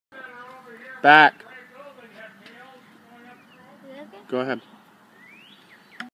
Throwing knife
My youngest daughter throwing knives.